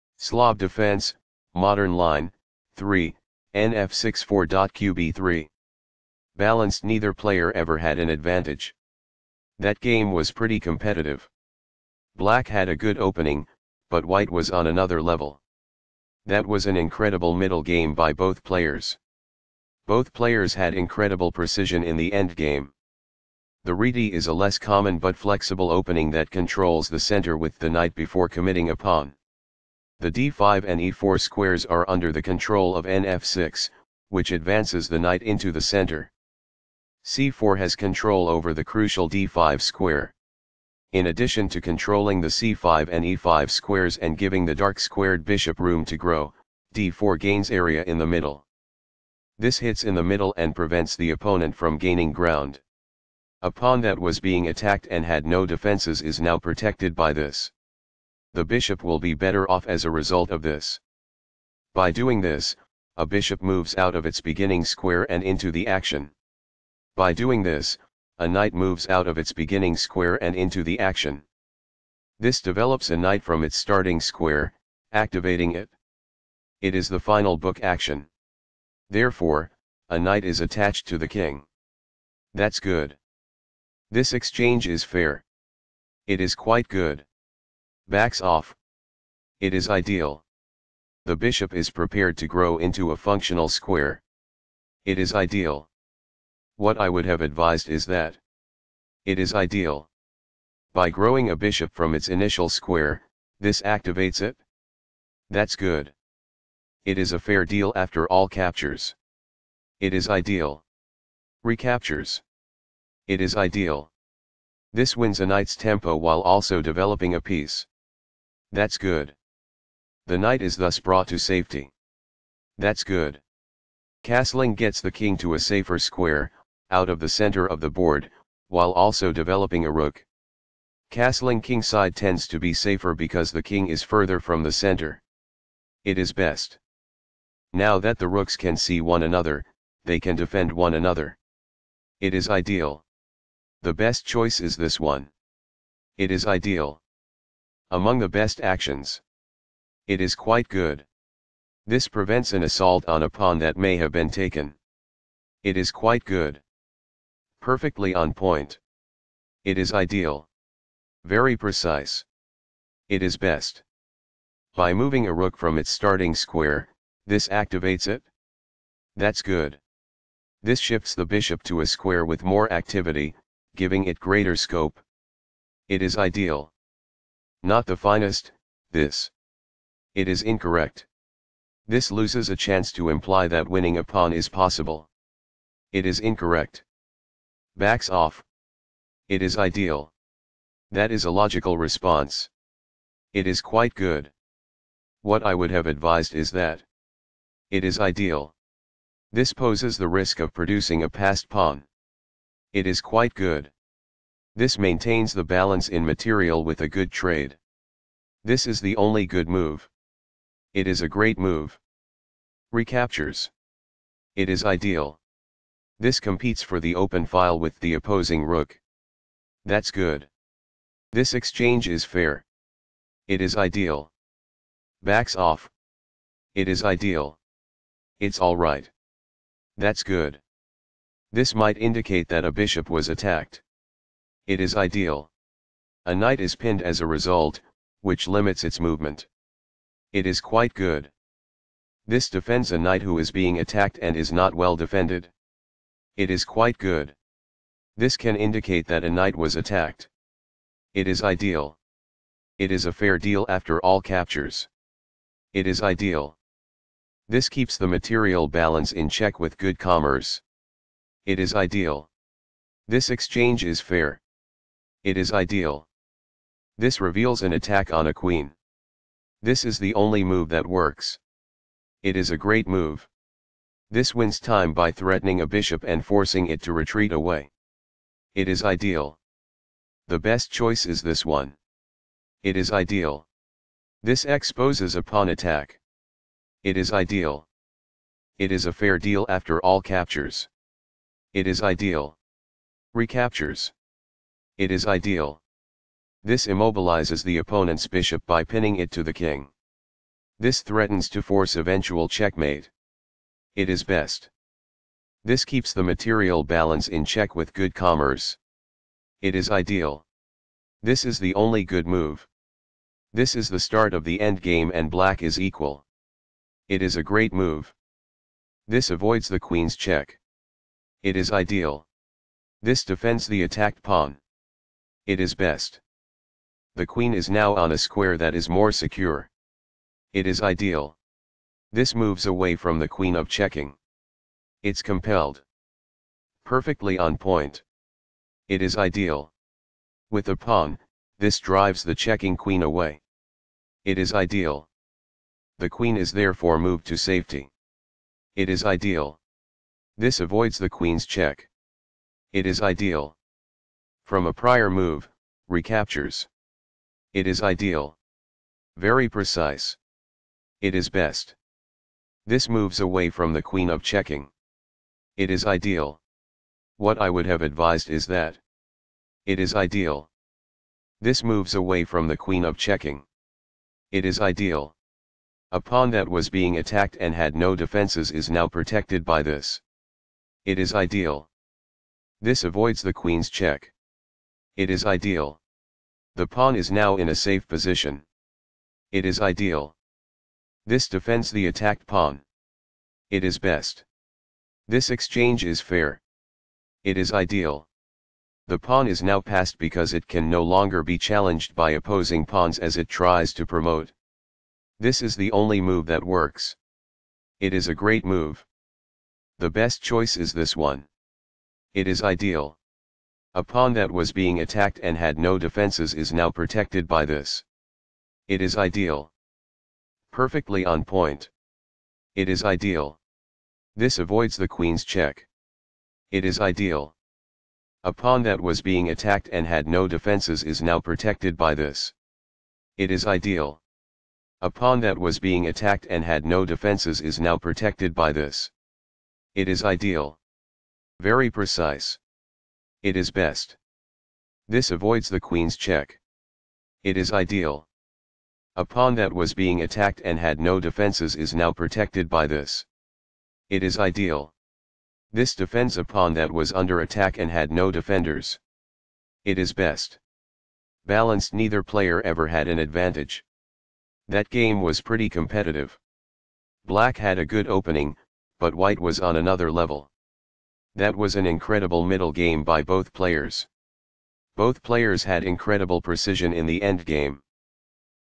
White Caruana, F., Black Kamsky, G.,Slav Defense: Modern Line, 3...Nf6 4.Qb3, Event 40th TCh-GRE 20
Slav Defense: Modern Line, 3...Nf6 4.Qb3, Event 40th TCh-GRE 2012, Site Achaea GRE, Date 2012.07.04, Round 6.1, White Caruana, F., Black Kamsky, G., Result 1/2-1/2, ECO A15, WhiteElo 2770, BlackElo 2741, PlyCount 87, EventDate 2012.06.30, EventType team, EventRounds 7, EventCountry GRE, Source Mark Crowther, SourceDate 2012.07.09, WhiteTeam CP PERISTERI, BlackTeam CN Kavala